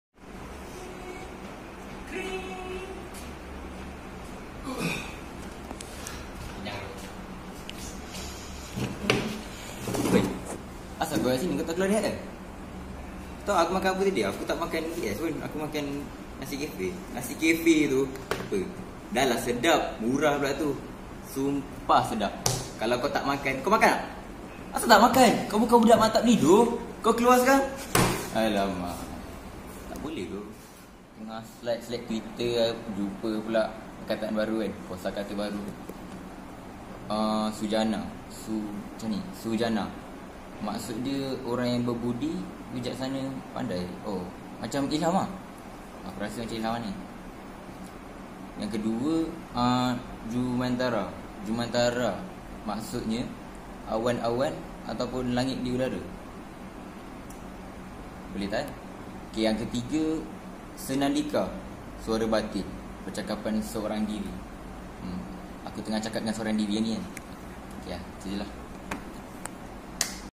Kosa kata menarik
Kepelbagaian kosa kata menarik dalam memperkasakan kemahiran mengarang karangan bahasa melayu